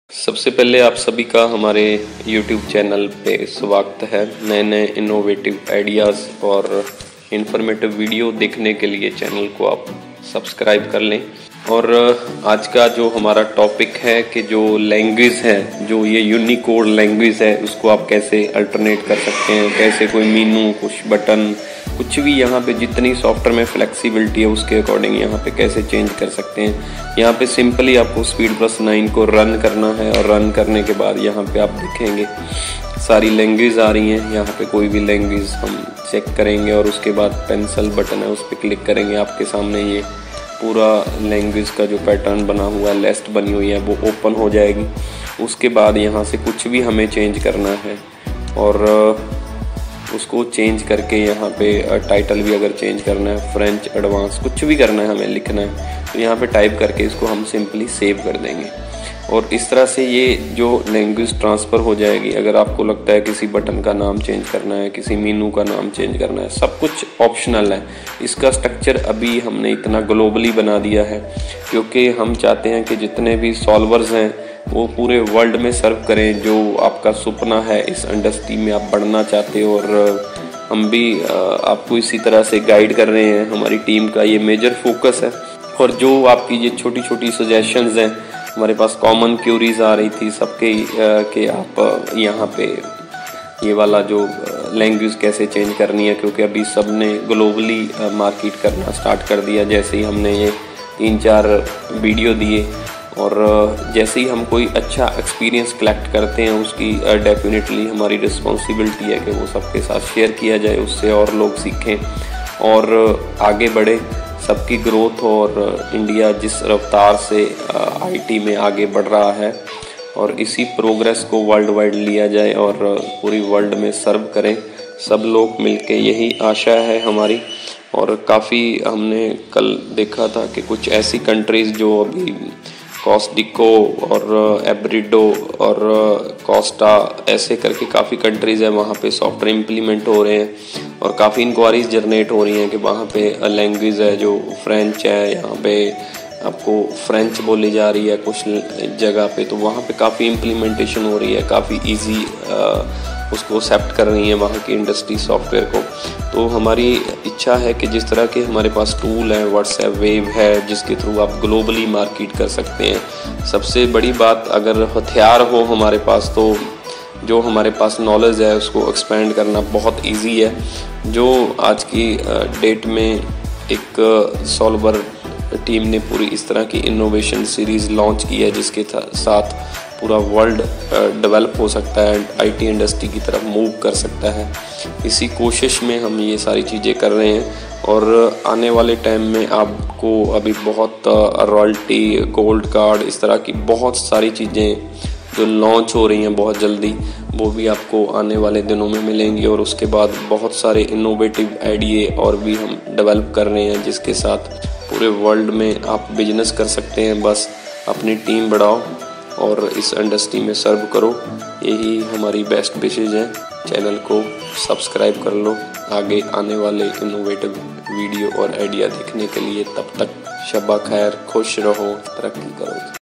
How Change Language in Speed Plus 9.0 Unicode ERP Software For worldwide market in every country.
Expand your boundries, generate business from every country. Gain Name, fame, money by implementing Speed Plus 9.0 Multi Language Software worldwide.